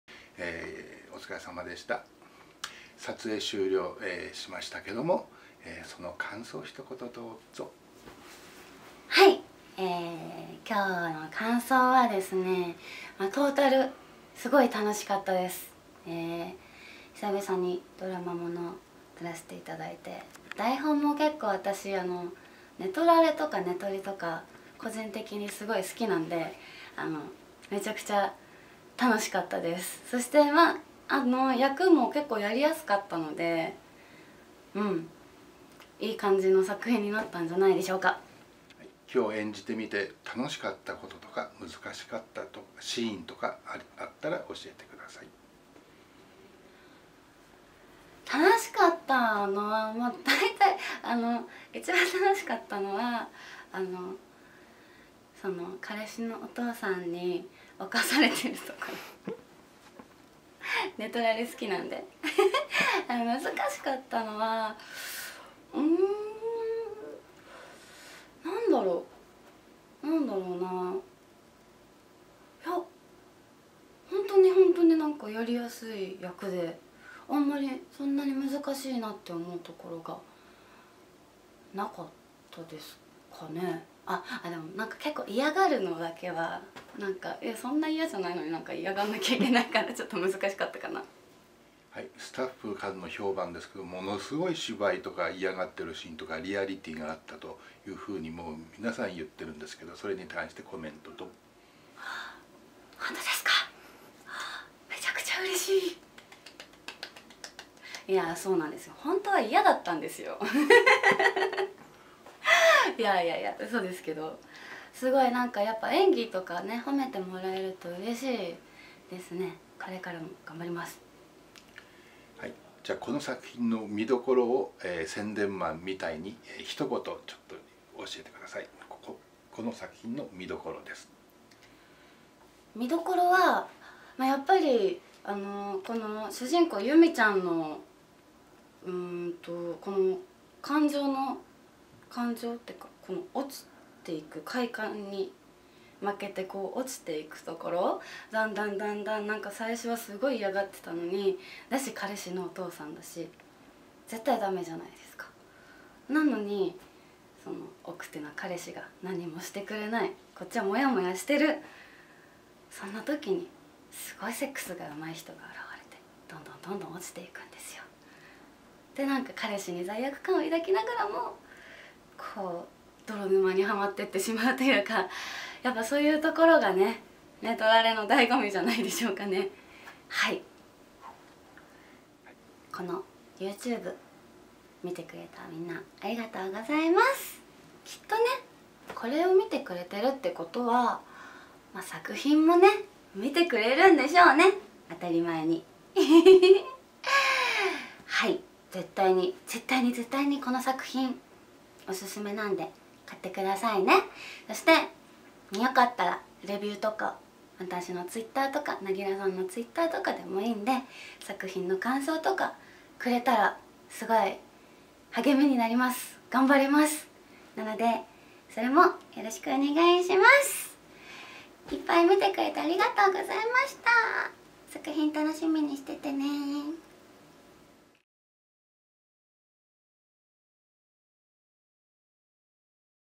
紺野ひかるさんドラマ作品インタビュー｜撮影現場生中継＆本格ドラマ作品 NAGIRAチャンネル
紺野ひかるさん　ドラマ作品
いよいよリリースされました！

ここだけの舞台裏のお話をインタビュー形式で。

嫌じゃないってのが、笑えます～(^^)

『ドラマ撮影の舞台裏、ドキュメンタリーを動画でアップ中！』